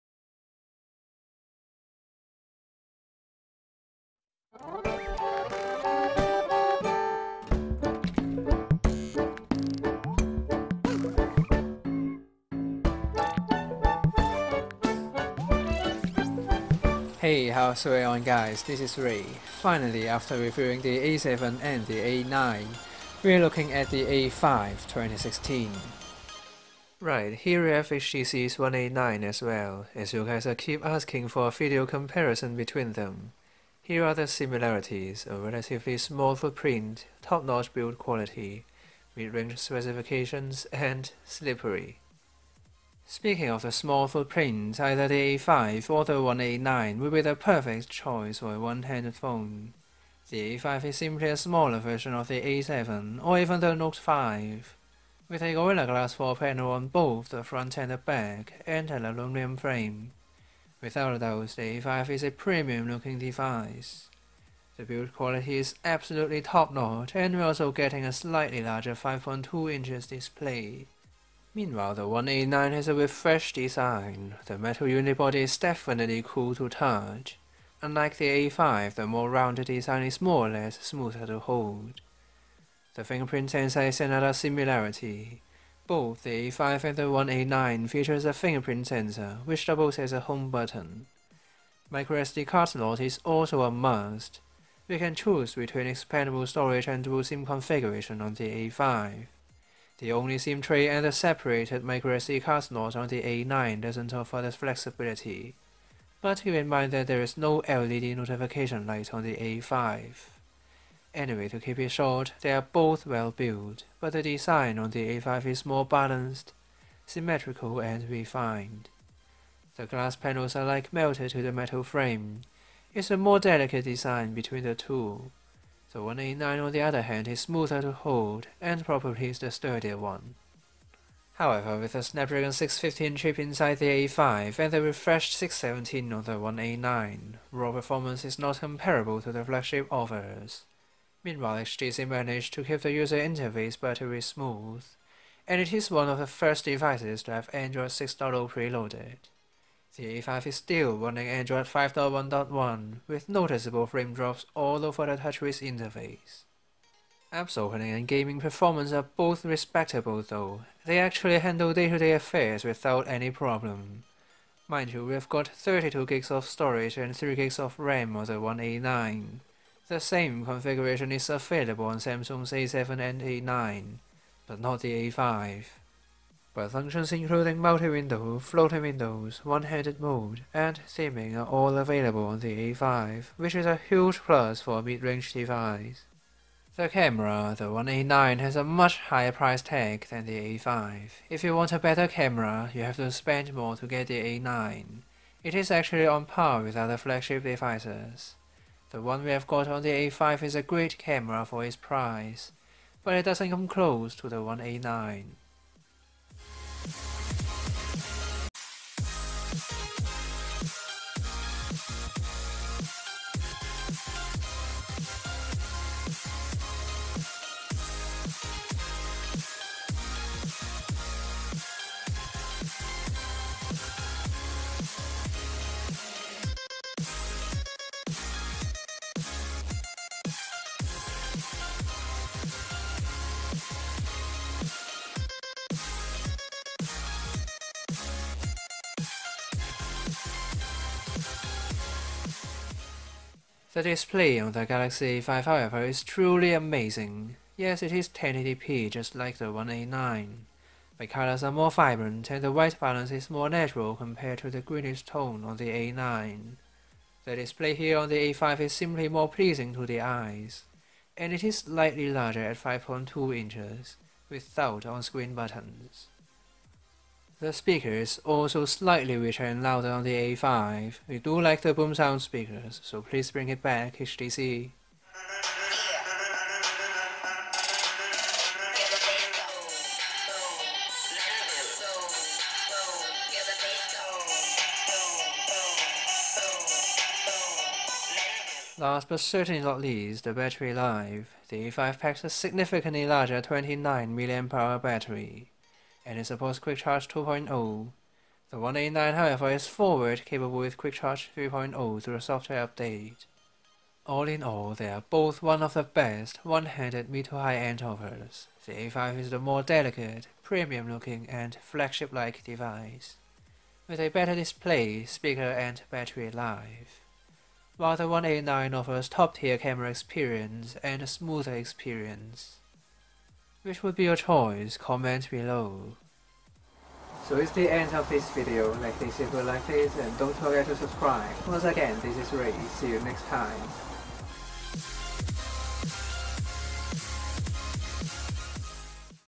Samsung Galaxy A5 (2016) Review! feat. HTC One A9
Finally, the in-depth review on the Galaxy A5 (2016) is here! Looking for a mid to high end device? It won't be amazing without the HTC One A9. Hope you guys enjoy this "Galaxy A5 (2016) vs HTC One A9" video!

Ray Ma @FlashingDroid TV Presents